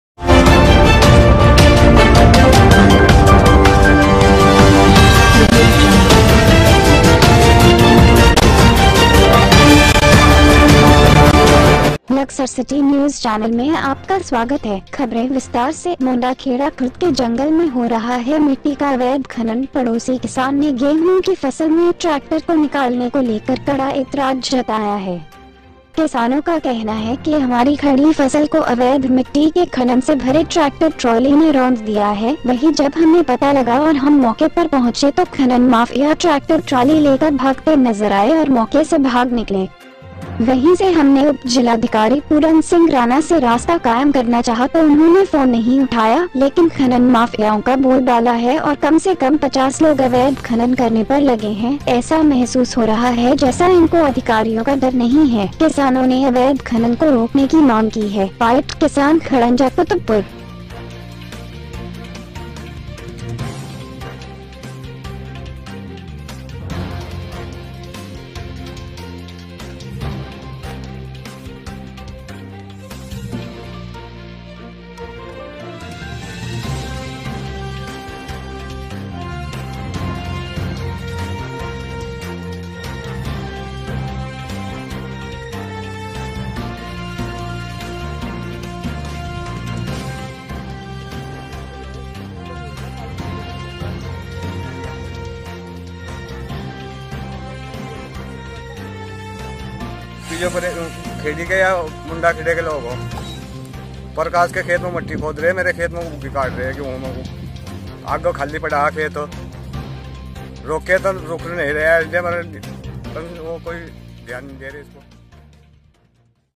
बिना परमिशन के चल रहा है अवैध खनन किसान के गेहूं तोड़कर निकाला ट्रैक्टर ट्राली
ब्यूरो दिलशाद अली आर्य कंपलेक्स बालावाली रोड लक्सर